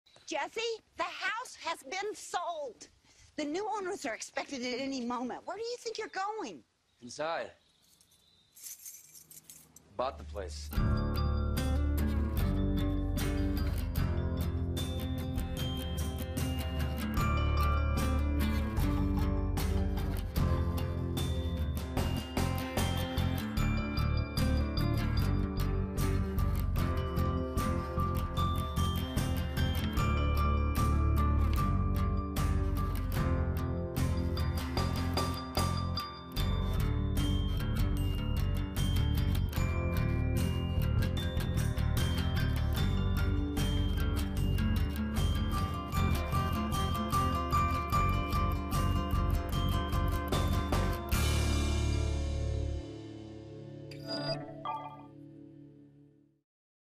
'Breaking Bad' house listed for 1.6M
Ever since Jesse Pinkman (from AMC's "Breaking Bad") took over his parents' home, we have all wanted to settle down in this Albuquerque, New Mexico, house. Well, now you probably can. If you have $1.6 million, this house could be yours!